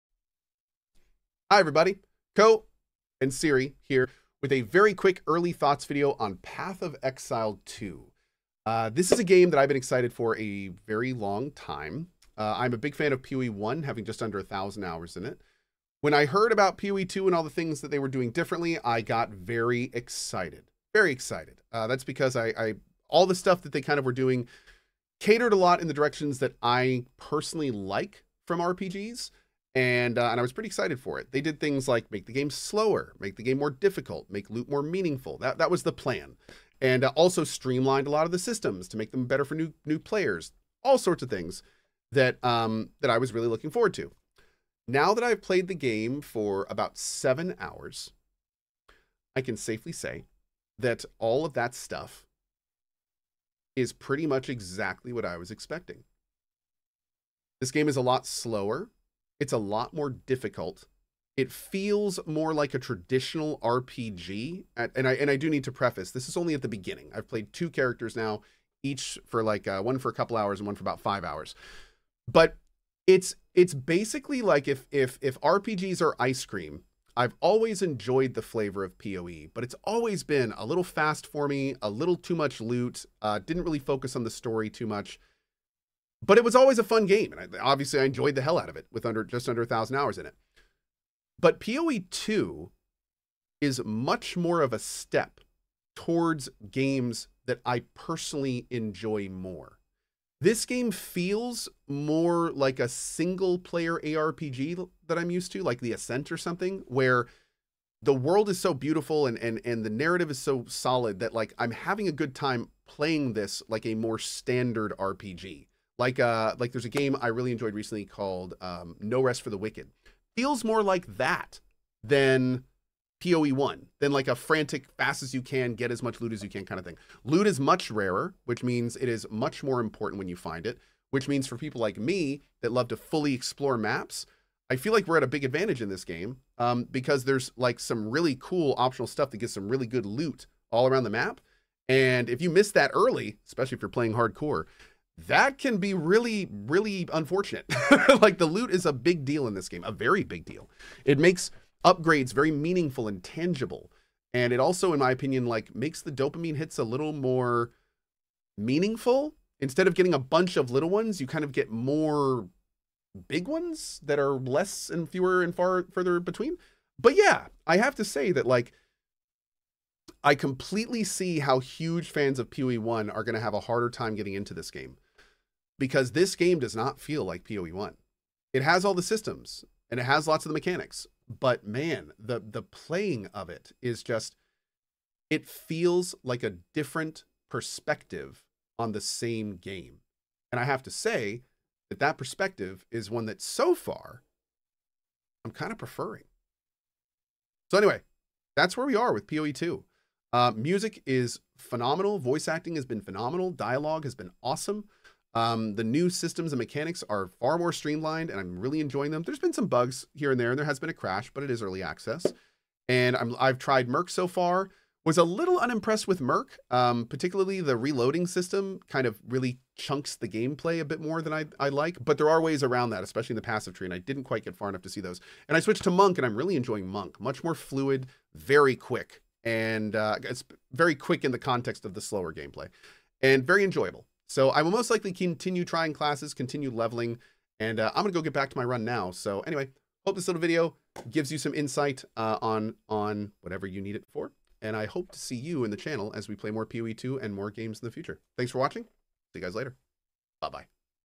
Cohh's Early Thoughts On Path Of Exile 2 Early Access
Cohh talks about his Early Thoughts on Path Of Exile 2 after playing for around 7 hours.